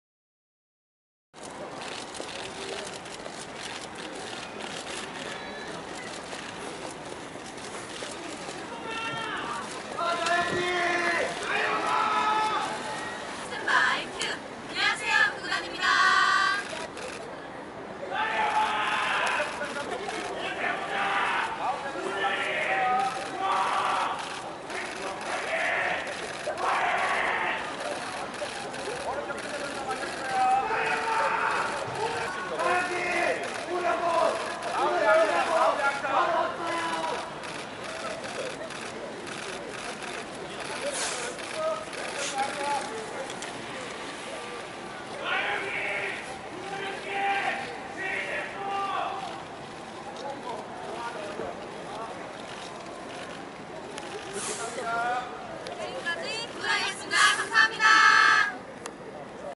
[뉴스인스타] 171124 구구단 KBS 뮤직뱅크 출근길
171124 구구단 뮤직뱅크 리허설 출근길 풍경 입니다.

GATE 9, KARD, NRG, SF9, 빅톤(VICTON), 구구단, 더 로즈, 더 유닛, 러블리즈, 몬스타엑스, 블라블라, 사무엘, 세븐틴, 소나무, 아스트로, 이하린, 트와이스, 펜타곤, 해시태그가 참여했습니다.